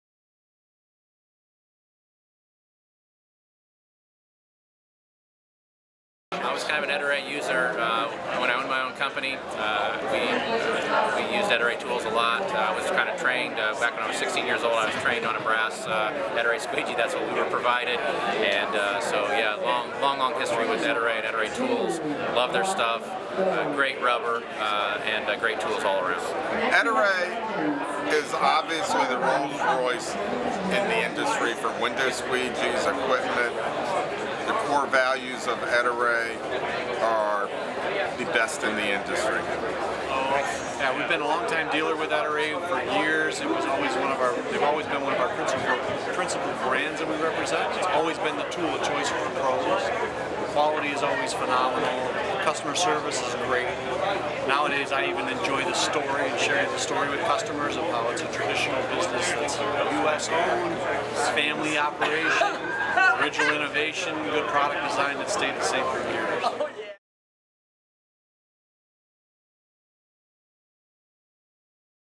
Ettore Customer Testimonials (Part 3)
Listen as window cleaning professionals attending the 2019 International Window Cleaning Association (IWCA)'s Annual Convention in San Diego, California, talk about what makes Ettore Products Company their choice for professional window cleaning tools and accessories.

Featured:
Michael Draper, American Window Cleaner Magazine
Kenny Cohn, Kevco Building Services
Michael Waroway, Detroit Sponge & Chamois